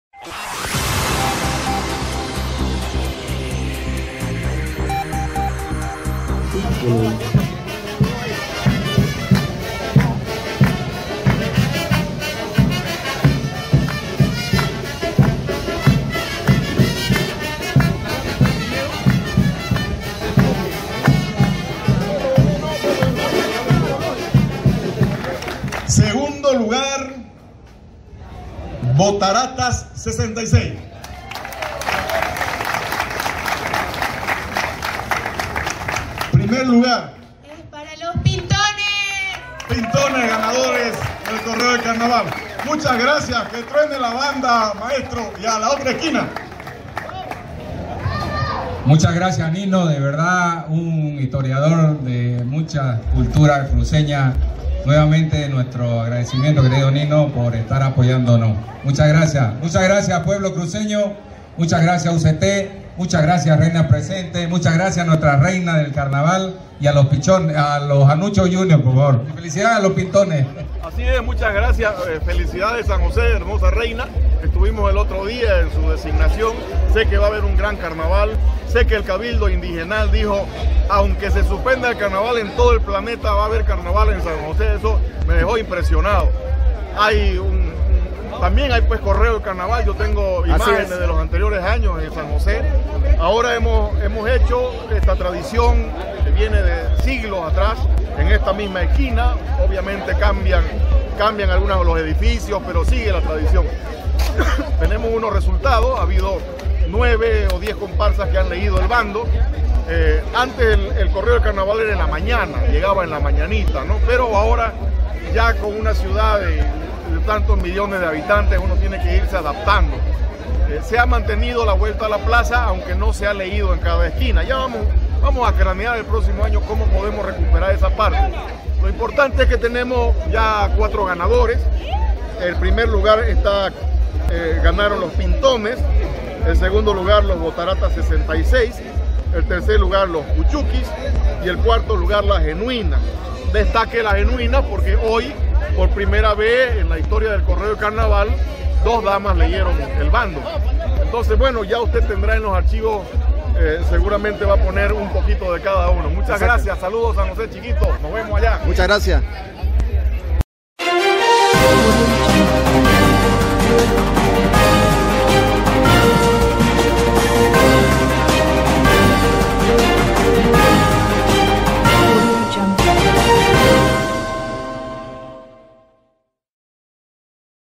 Los Pintones elaboraron el mejor bando carnavalero de Santa Cruz
La tarde de este Domingo, se procedió a la lectura del tradicional Bando Carnavalero Cruceño, actividad que se lo implementa desde más de 4 Siglos atrás y que en esta oportunidad fue organizada por la ACCC.
El historiador Nino Gandarillas, quien cumplió el papel de Jurado Calificador, fue el encargado de anunciar los ganadores.